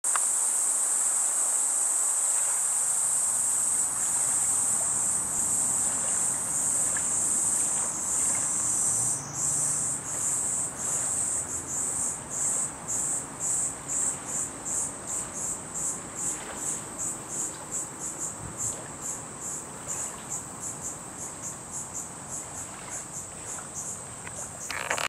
Sound and the shores of the waves 波の音・琵琶湖013
Sound and the shores of the waves　波の音・琵琶湖湖岸
140714oumihachimann013
Lake Biwa,Shiga Ken Omihachiman,40-minute train ride from Kyoto
琵琶湖・滋賀県・近江八幡市,京都から電車40分